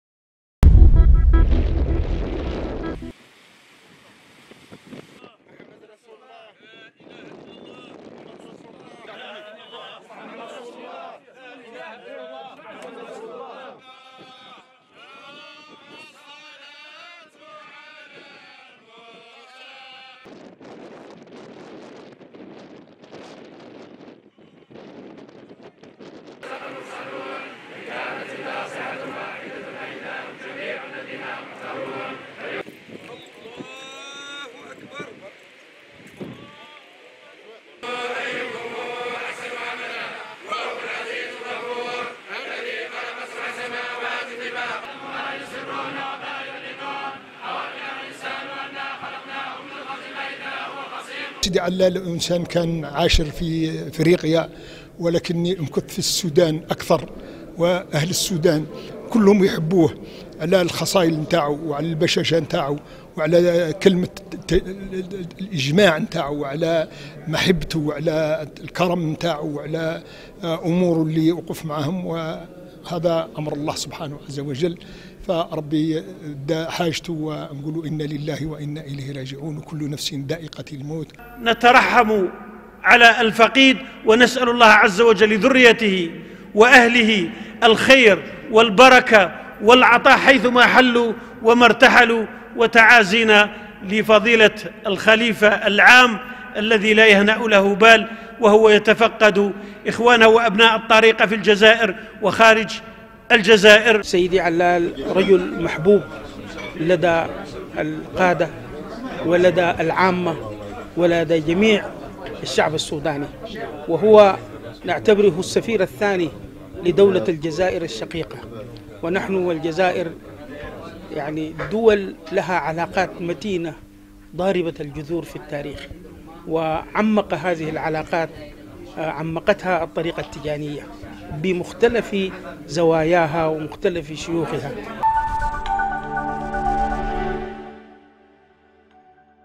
الأغواط: ممثل الخليفة العام للطريقة التيجانية بالسودان يوارى الثرى بعين ماضي
الأغواط   (وأج)- شيعت بمقبرة قصر كوردان بعين ماضي (70 كلم غرب الأغواط) جنازة ممثل الخليفة العام للطريقة التيجانية بدولة السودان، الشيخ علال بن حميدة التيجاني، بعد أن وافته المنية يوم 30 مارس الماضي بالخرطوم عن عمر ناهز 77 سنة.